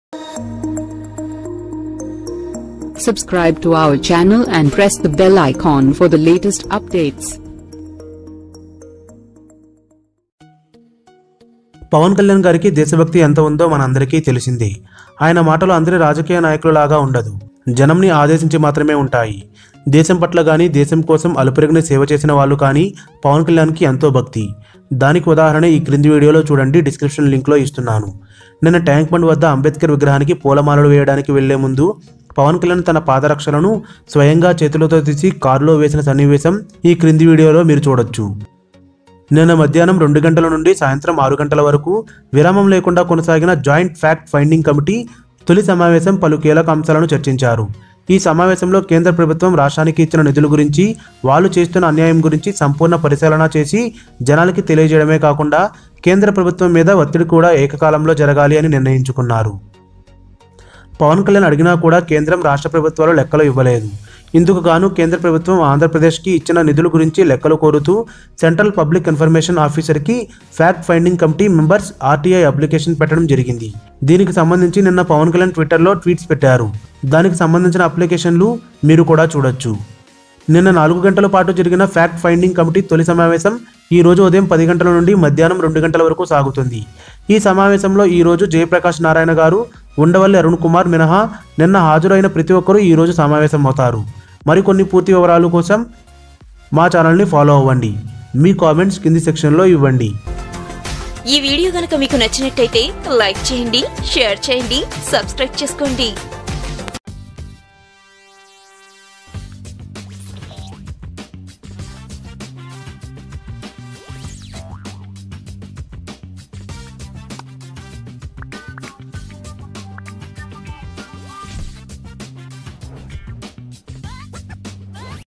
OMG! See How Pawan Kalyan Did A Great Thing Hatsoff | Ambedkar Statue At Tank Bund | Film Mantra
OMG! Pawan Kalyan Takes His Slippers With Hand while going to Ambedkar Statue At Tank Bund.